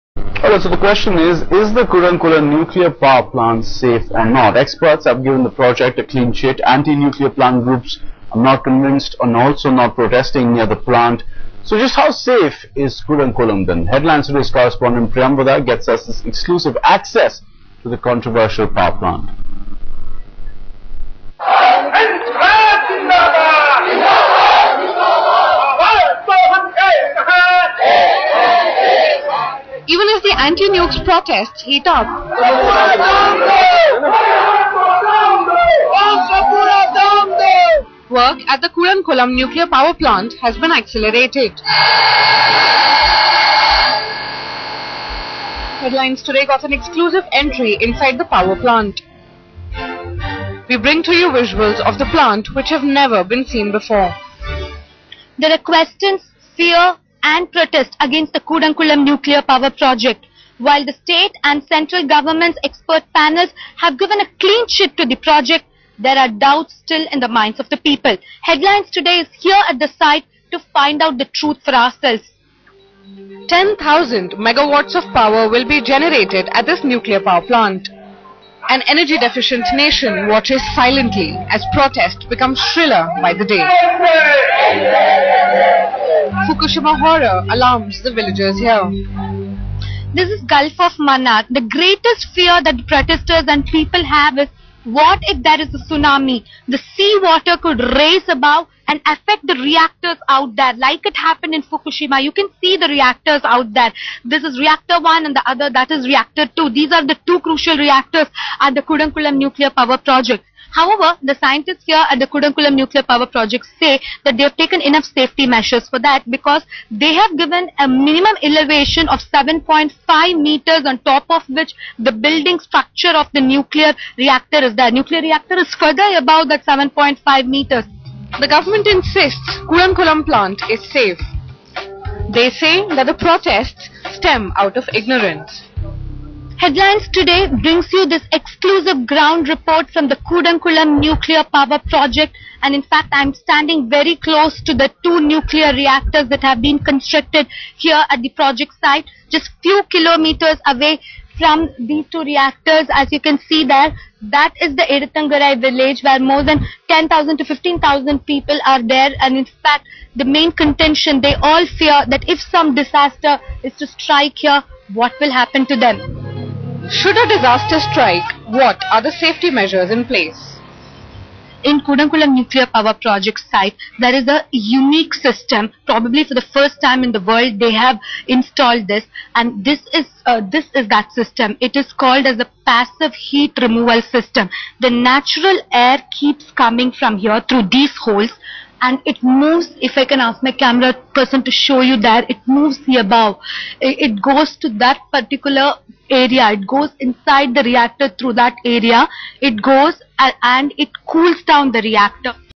How safe is Kudankulam nuclear plant? Part 1 of 2
Headlines Today brings exclusive ground report from controversial Kudankulam nuclear power project.